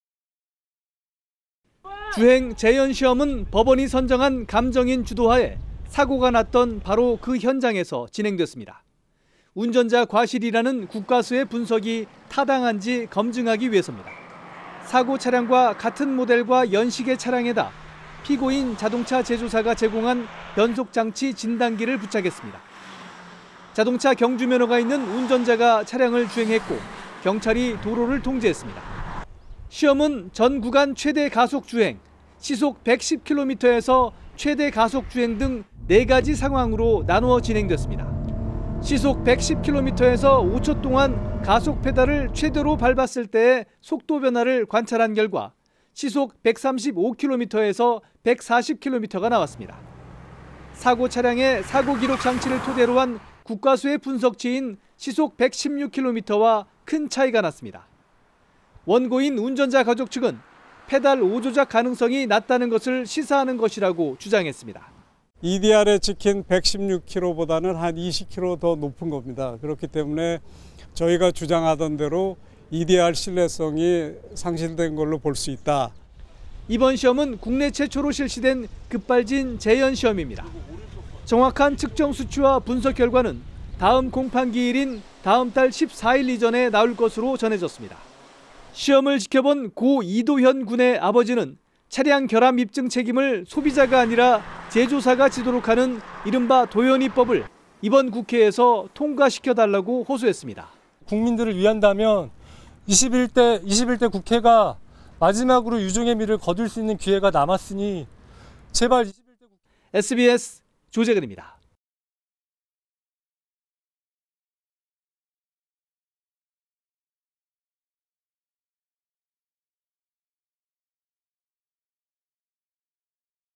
'강릉 급발진 의심 사고' 재연 시험…결과는? (자막뉴스) / SBS
강릉 급발진 의심 사고과 관련해 국내에서 처음으로 재연 시험이 이뤄졌습니다. 사고 유가족 측이 수억원의 비용까지 부담하면서 요구한 걸 재판부가 받아들여 이뤄졌습니다.